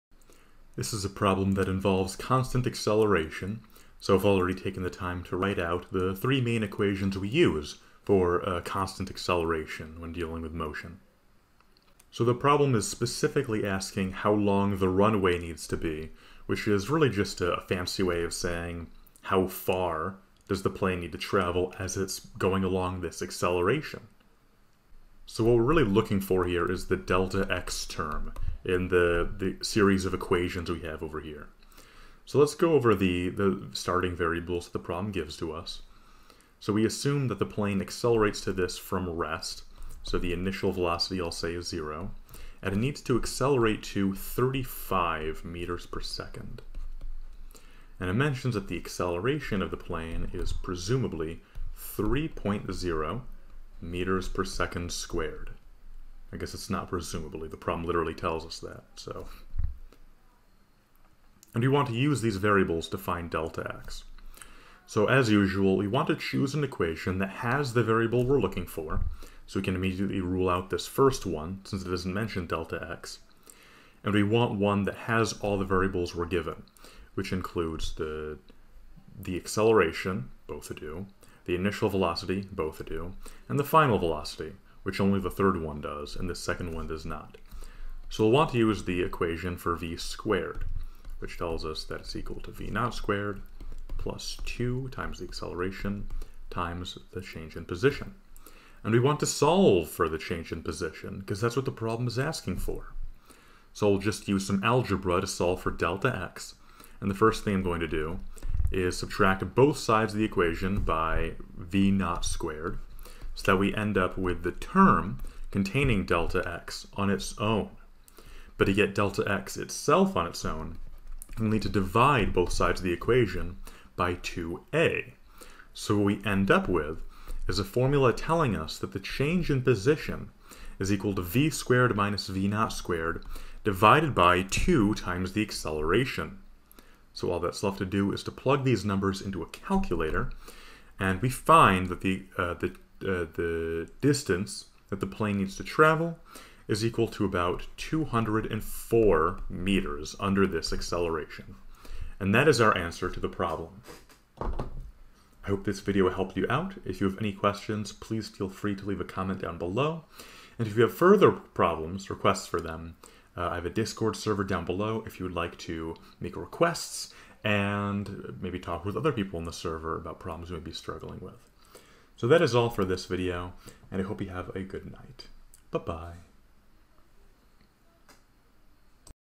A light plane must reach a speed of 35 m/s for takeoff. How long a runway is needed if the
A light plane must reach a speed of 35 m/s for takeoff. How long a runway is needed if the (constant) acceleration is 3.0 m/s^2?

Even a small amount of money can add up and support me immensely!